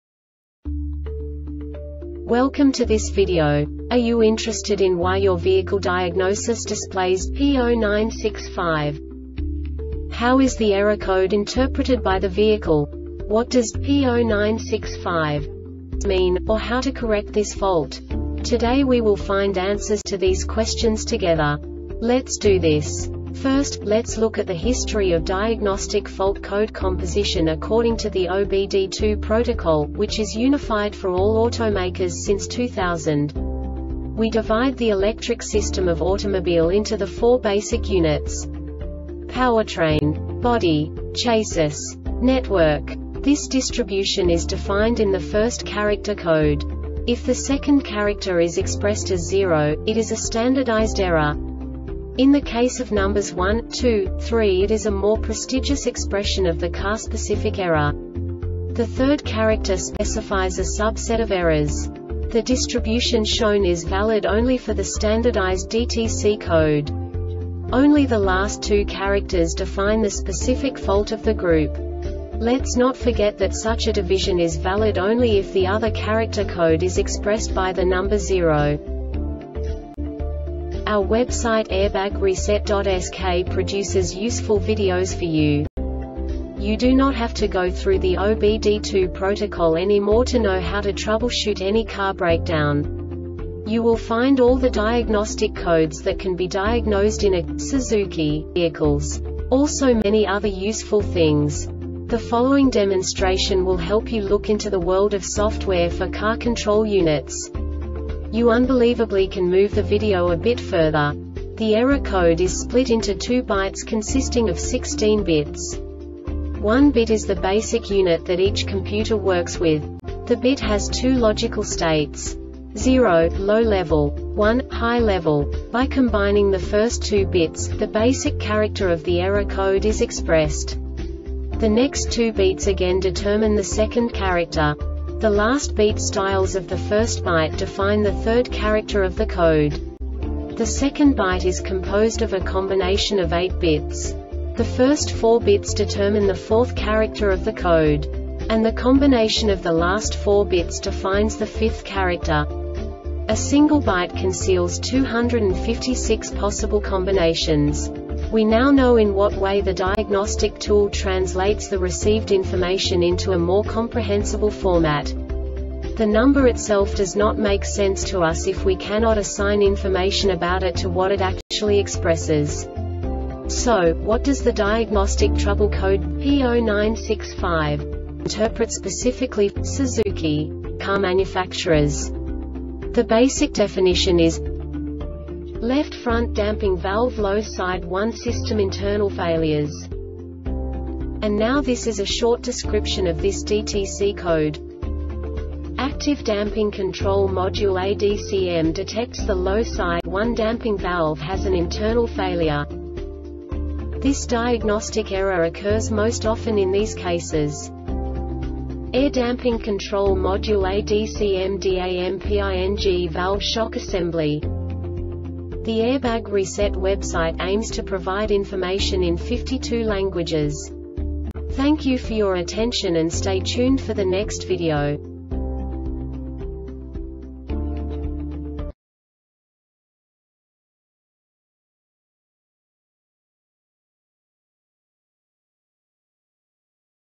DTC Suzuki P0965 Short Explanation
Contents:
0:21 Basic DTC analysis according to OBD2 protocol standard.
1:48 Insight into programming
2:58 A diagnostically understandable description of the Diagnostic Trouble Code
3:05 Basic error definition

Make:
Suzuki
Code:
P0965
Definition:
Left Front Damping Valve Low Side 1 - System Internal Failures
Description:
Active Damping Control Module (ADCM) detects the Low Side 1 Damping Valve has an internal failure.
Cause:
AIR DAMPING CONTROL MODULE (ADCM)DAMPING VALVE SHOCK ASSEMBLY